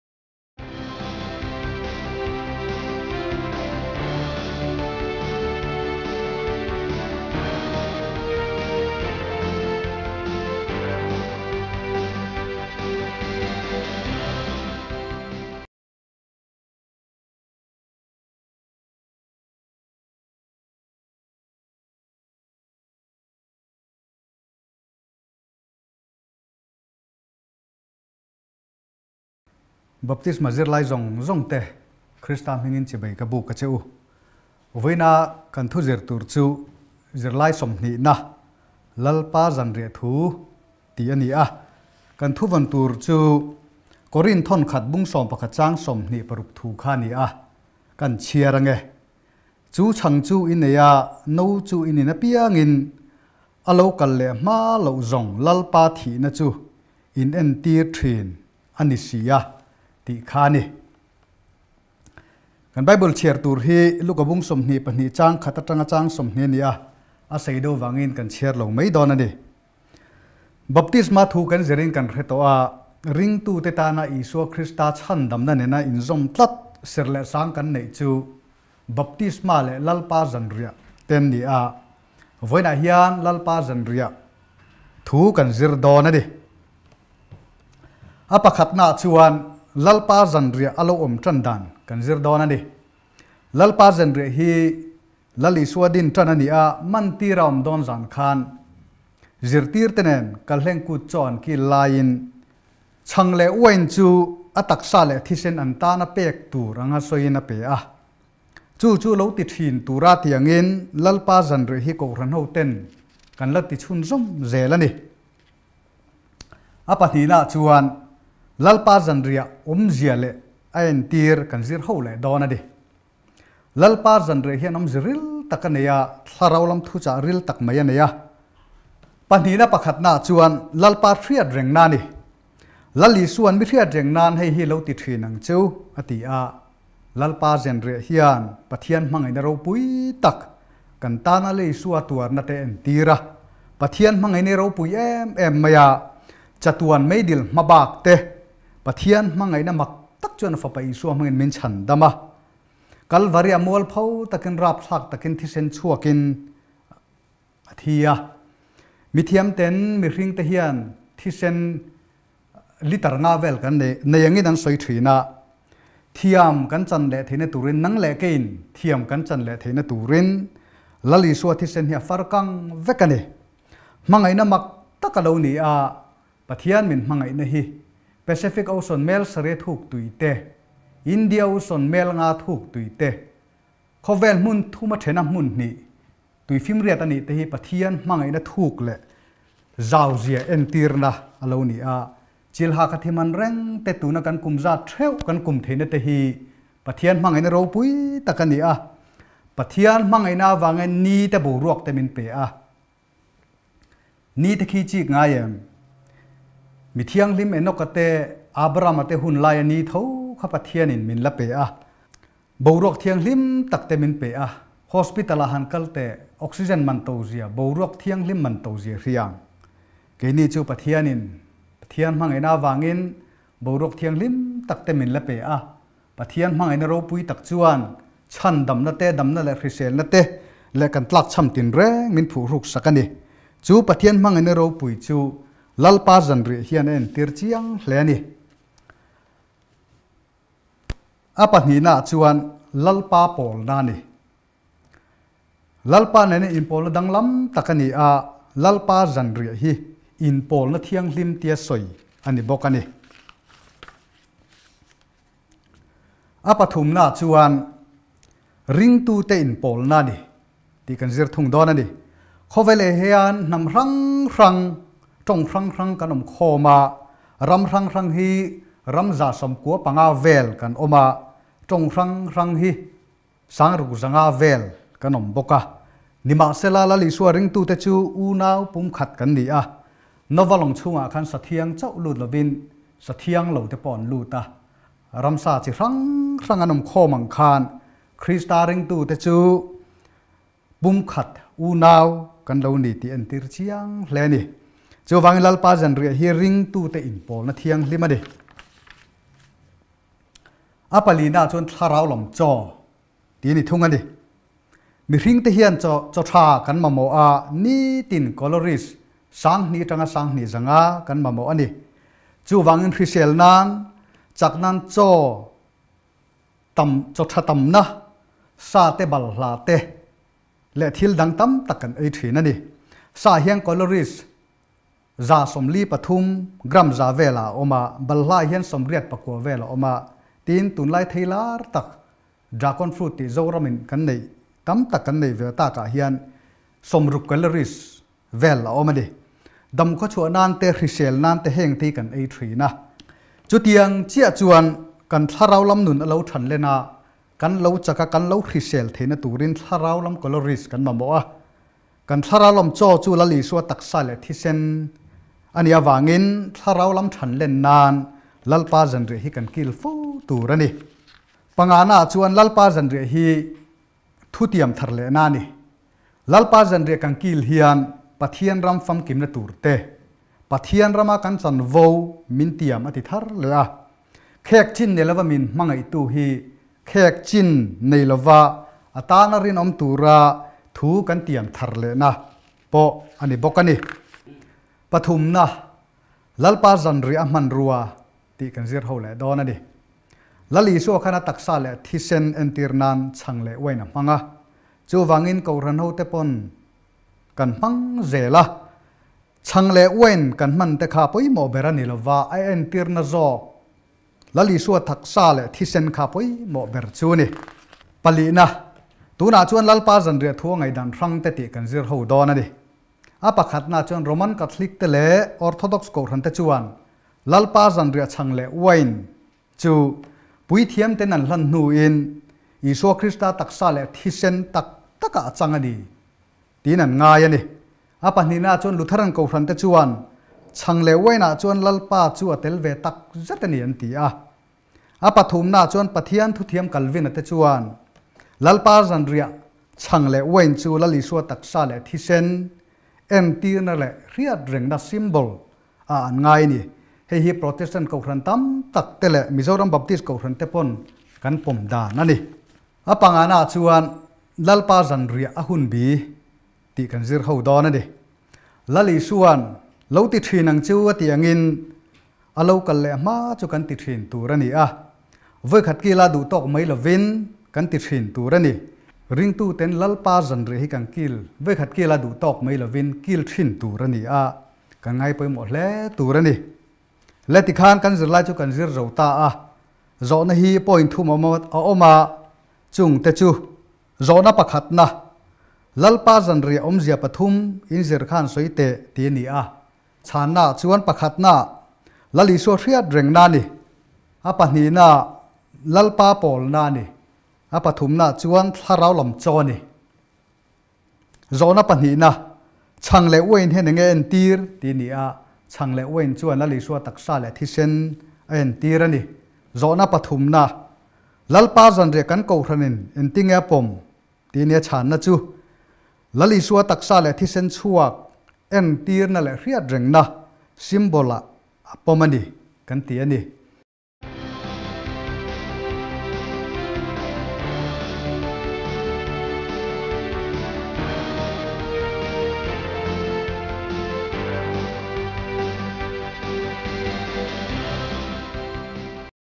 BCM Sunday School : Baptisma - Zirlai -20-na : (August 22, 2021)
BCM Sunday School : Baptisma' -   Zirlai -20-na : (August 22, 2021) Zirtirtu hi  Rbt. R. Pachhnga, BCM Zohnuai Sunday School, Baptisma Zirtirtu a ni.